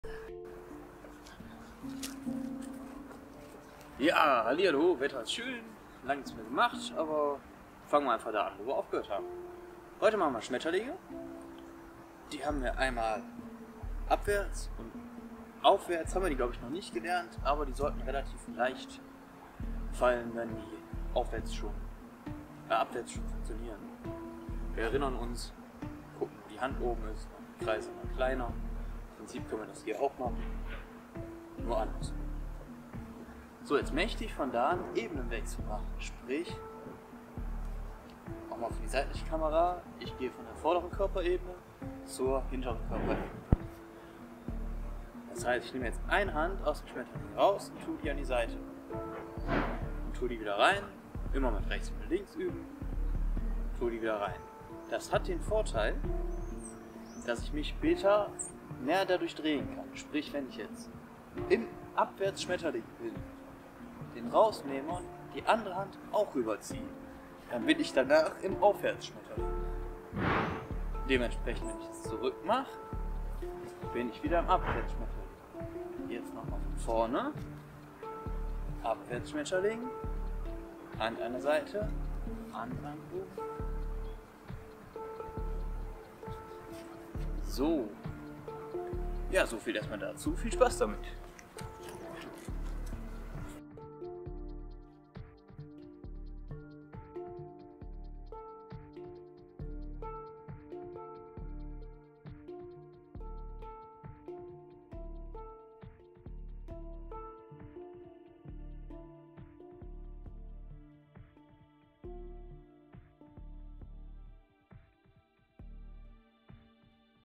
Poi Tutorial: Schmetterlinge und Ebenen
Eine Ergänzung zu Poi Schmetterlingen(Same time, entgegengestzte Drehrichtungen).  Eine Vorbereitung für Flowers ;)
Bei Fragen und Problemen könnt ihr euch gerne an mich richten.